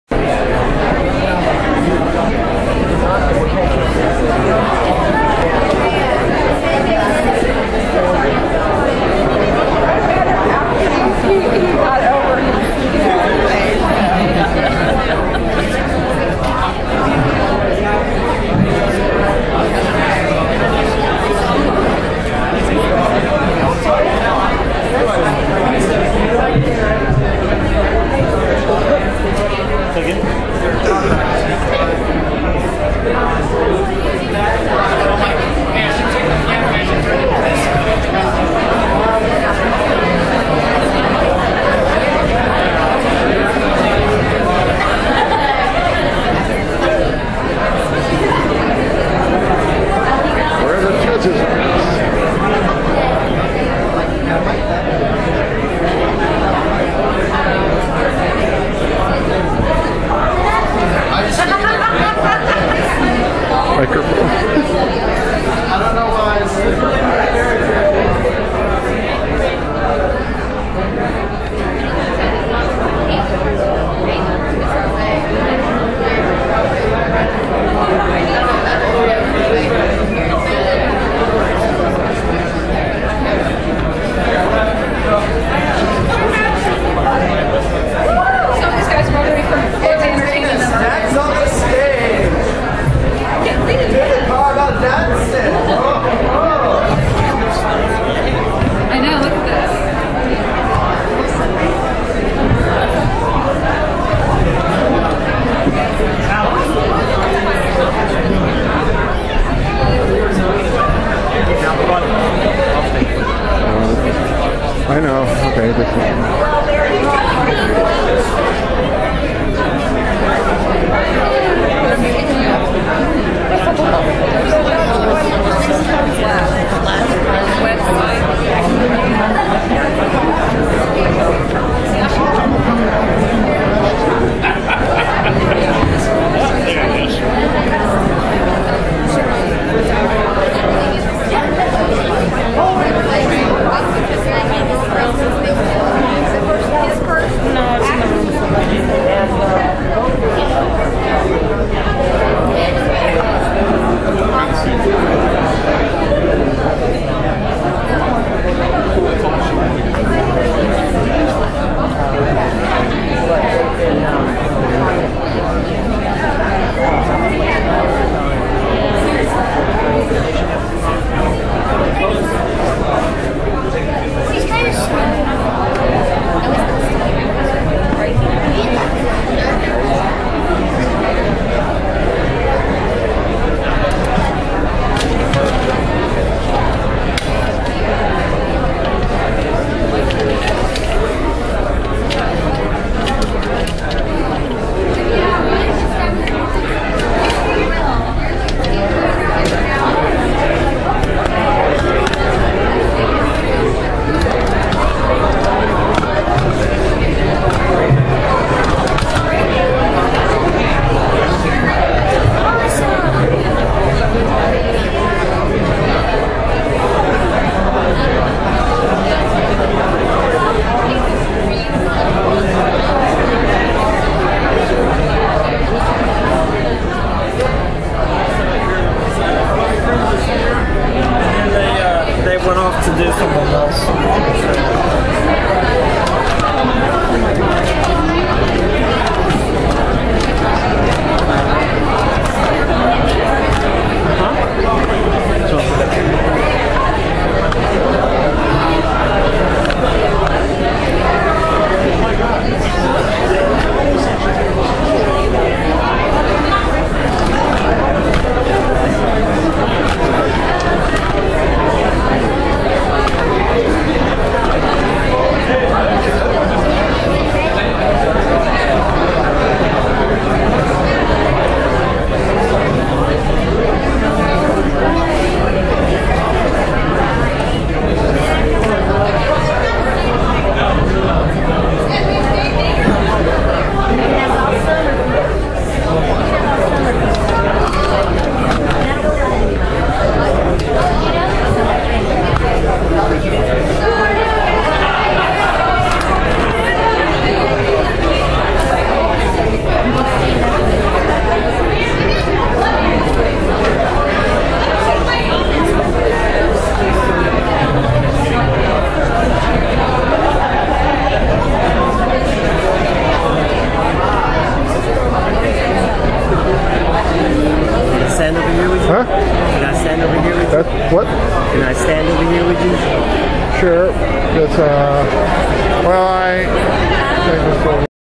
ALL-CON 2011 - Before Start of Costume Contest
Video footage of ALL-CON 2011 before the start of the
Costume Contest on Saturday evening.

Judges waiting to judge, introductions,
ALL-CON attendees, waiting for the event.